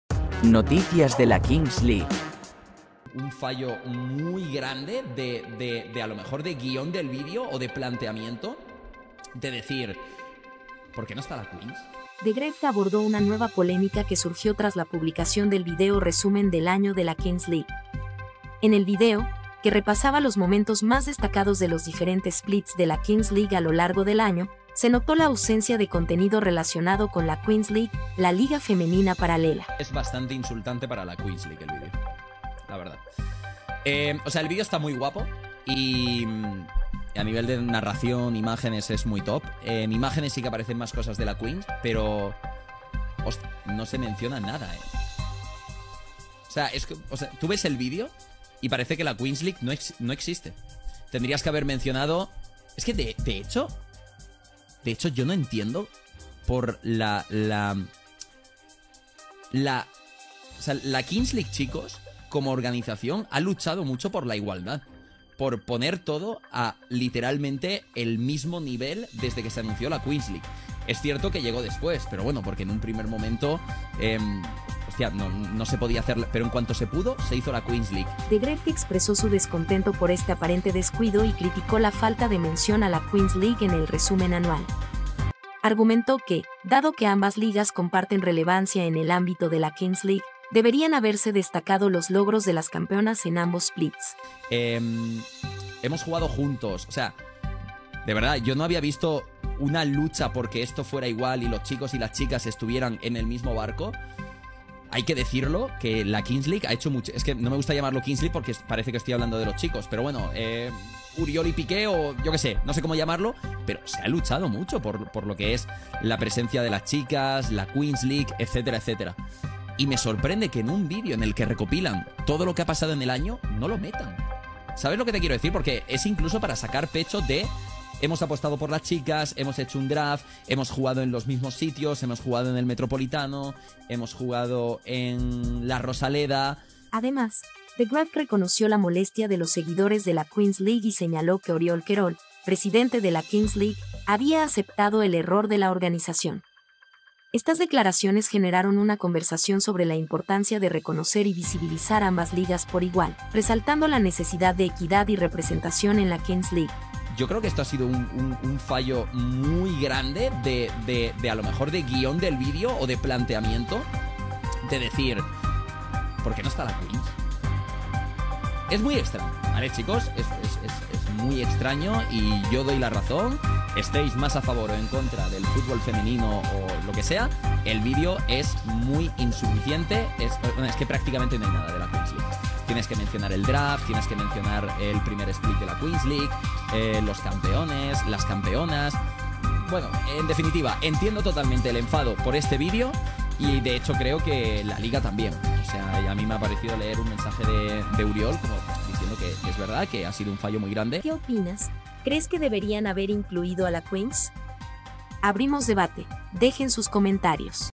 THEGREFG DEFIENDE A LA QUEENS LEAGUE TRÁS LA POLEMICA POR EL VIDEO RESUMEN
THE GREFG DEFIENDE A LA QUEENS LEAGUE TRÁS LA POLEMICA POR EL VIDEO RESUMEN
⚽JUEGO OFICIAL DE LA KINGS LEAGUE